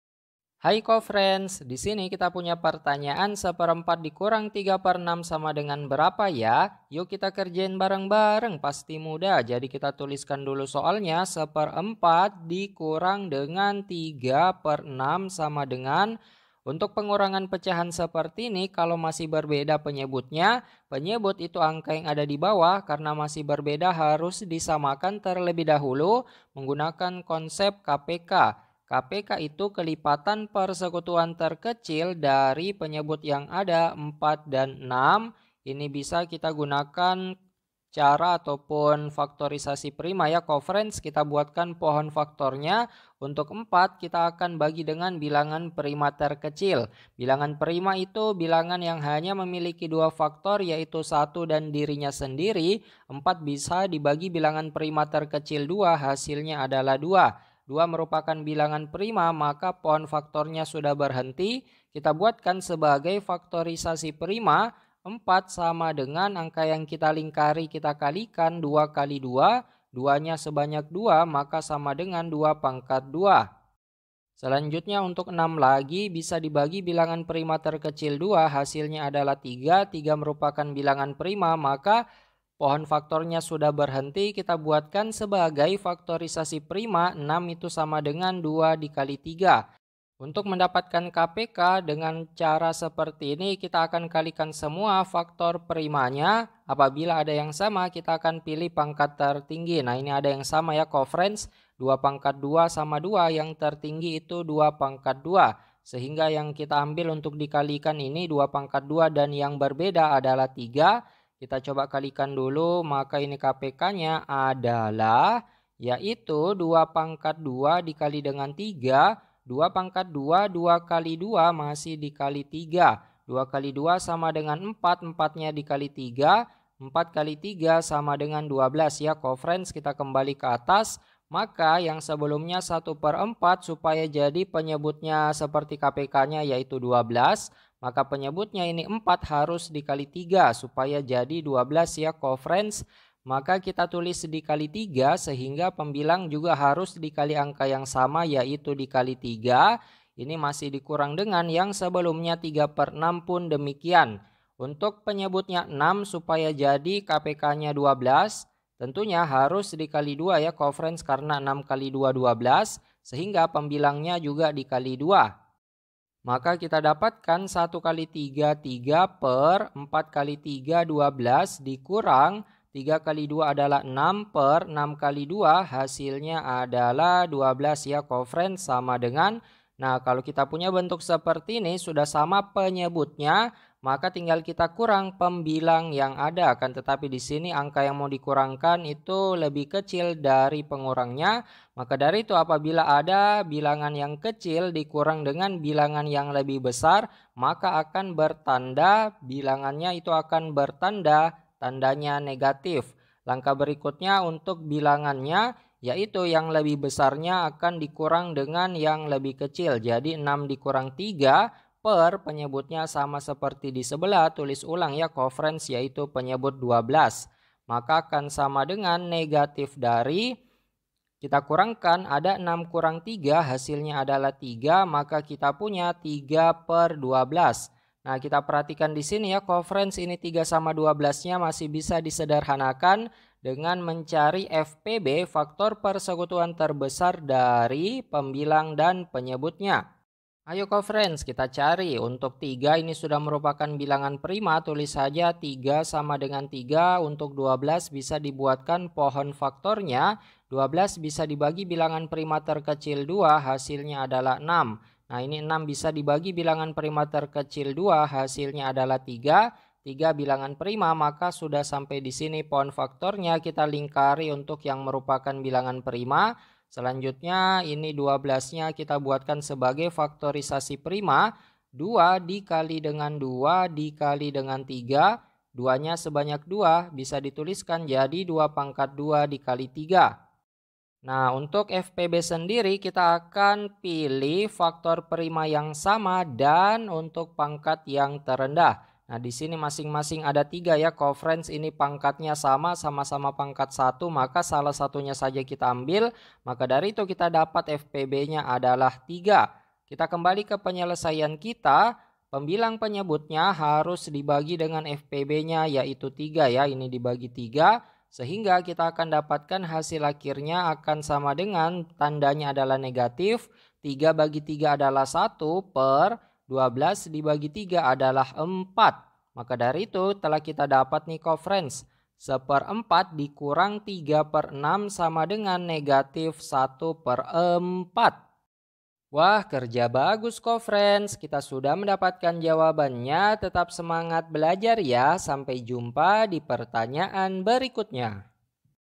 1 3 4 6
1 3 4 6

Topik atau Materi: Pengurangan Bilangan Pecahan - Operasi Bilangan Pecahan - Aritmatika - Matematika Kelas 5 SD -

Sumber Soal: Matematika SD dumatika.id , jeruq.com , majalahpendidikan.com  , N/A

00:00 : Bedah Soal
01:00 : Konsep, rumus dan pengertian Pengurangan Bilangan Pecahan
01:18 : Langkah penyelesaian soal
04:38 : Jawaban akhir
05:08 : Penutup